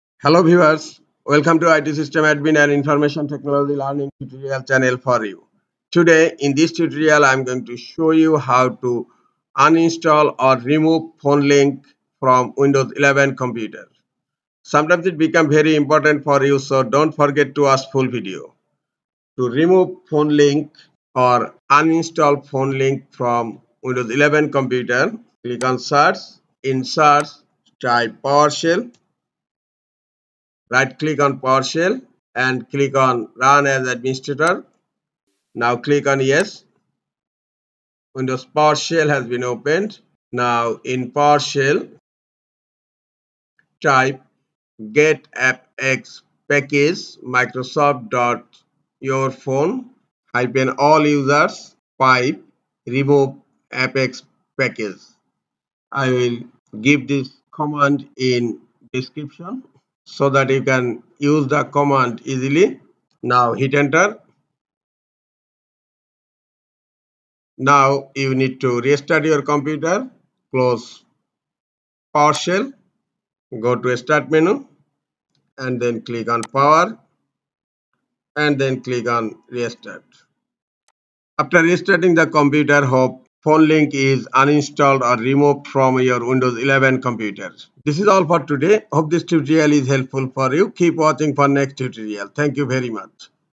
How To Remove or Uninstall Phone Link In Windows 11
Get-AppxPackage Microsoft.YourPhone -AllUsers | Remove-AppxPackage